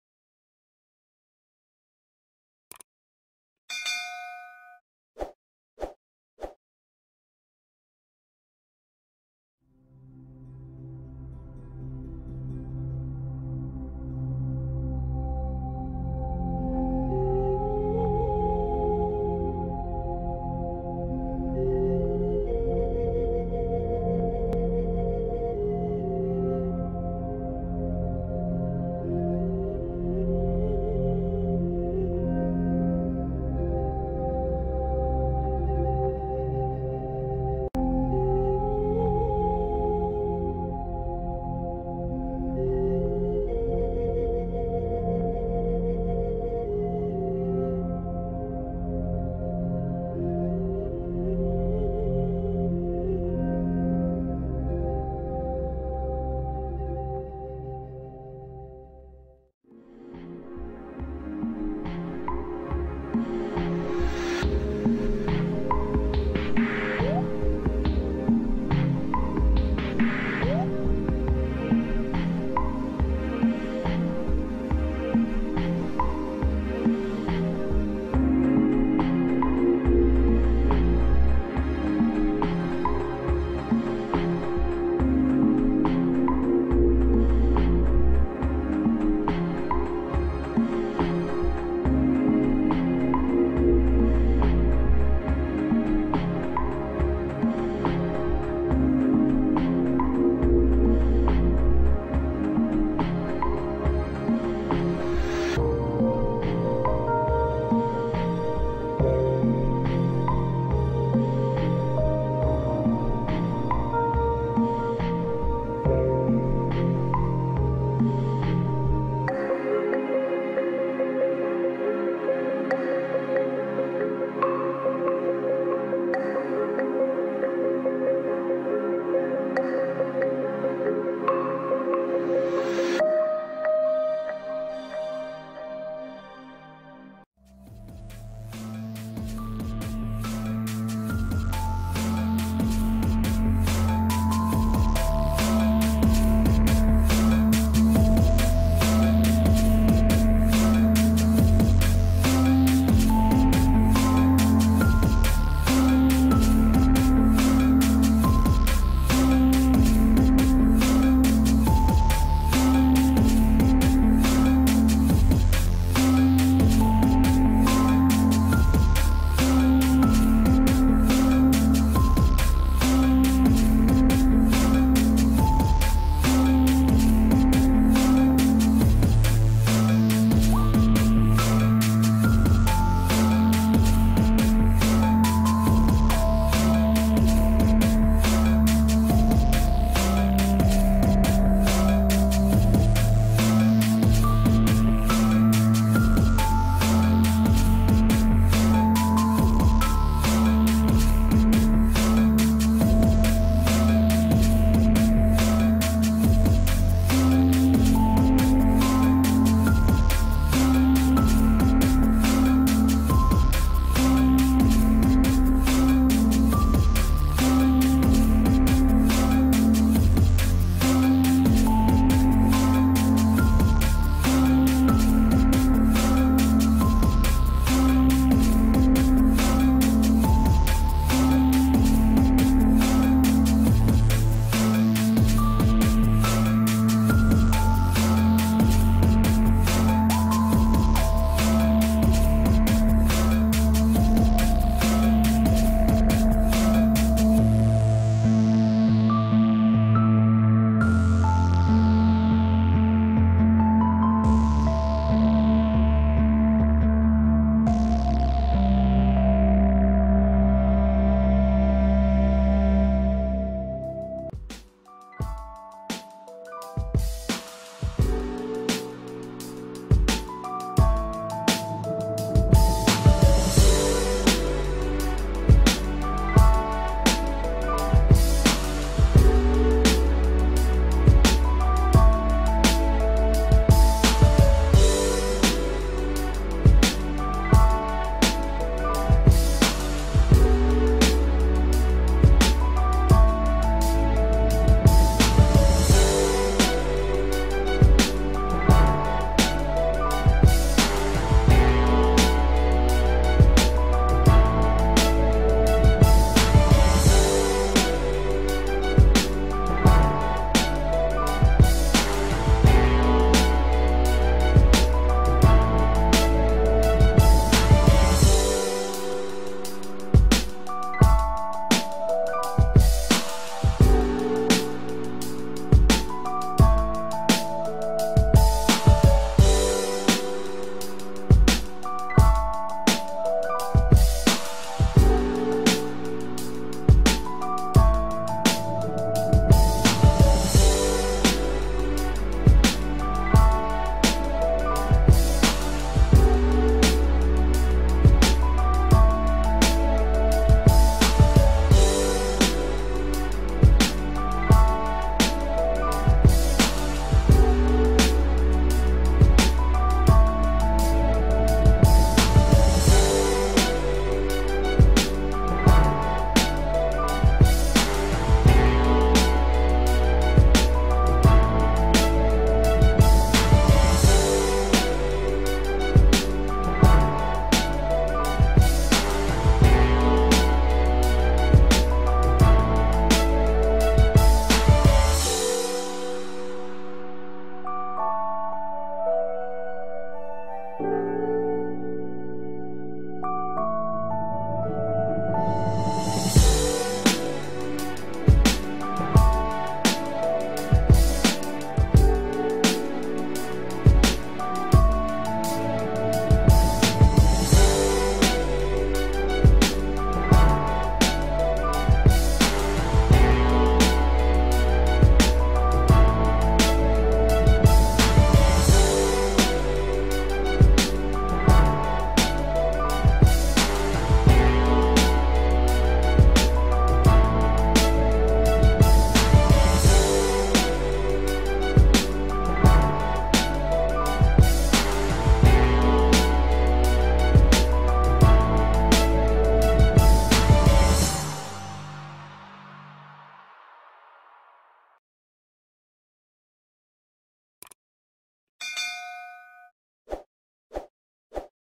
Lost Papyrus Reveals Extraterrestrials Visited Ancient Egypt
One of the most intriguing ancient texts, the Tulli Papyrus chronicles one of the earliest mass UFO sightings.

It also has an interesting backstory. In 1933, Alberto Tulli, director of the Egyptology section of the Vatican Museum, discovered a unique papyrus in one of Cairo’s antique shops. Unfortunately, it was priced too high—even for the Vatican—so Tulli resorted to copying down its contents.

يرجى الاشتراك. شكرا لكم